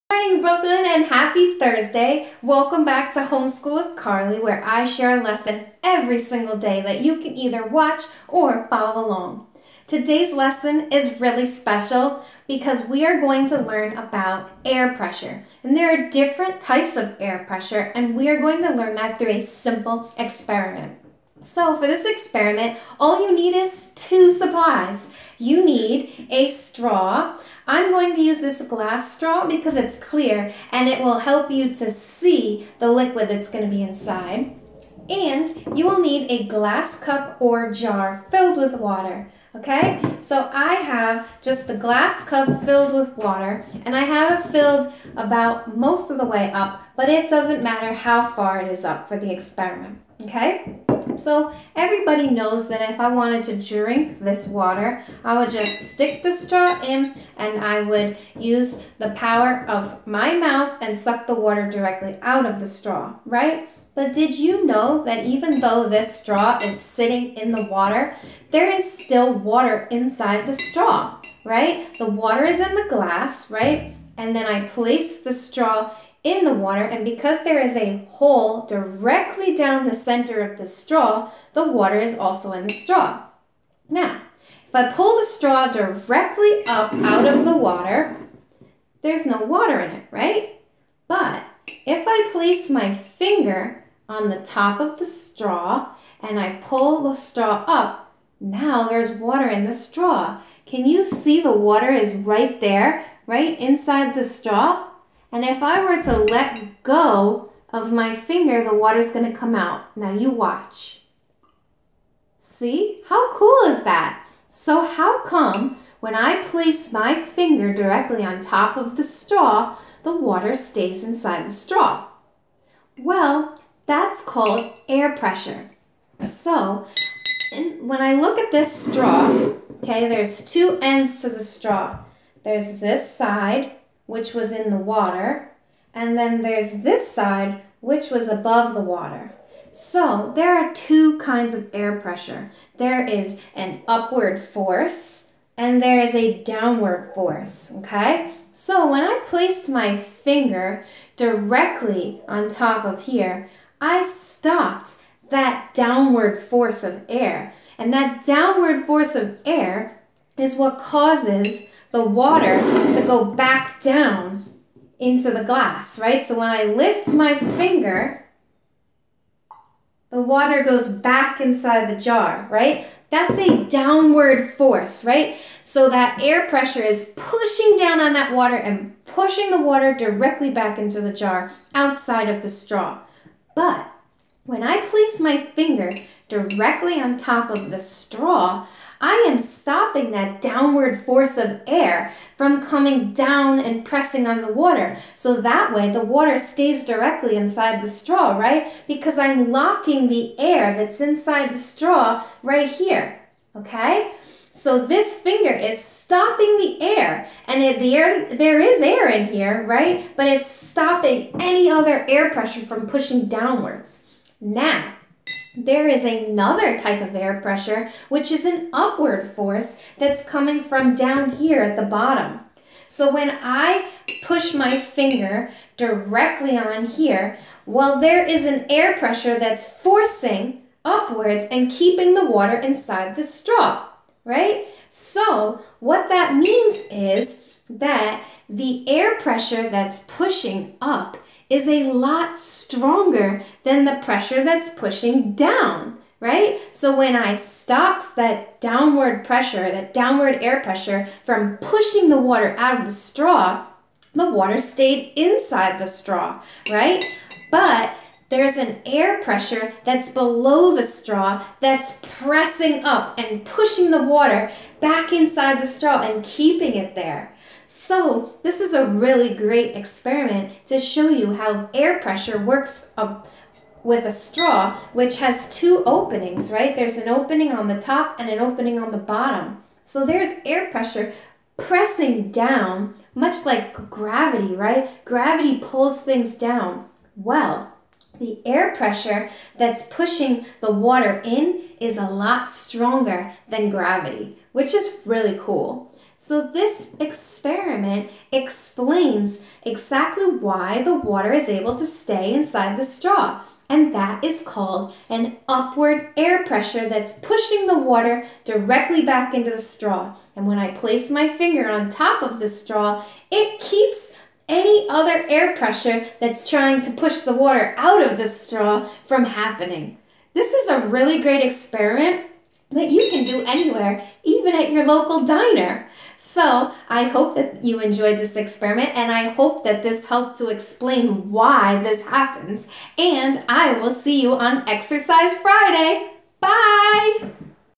Gravity VS Air pressure
In this activity you will see how air pressure is stronger than gravity in this specific experiment.

You need:
-glass full of water
-straw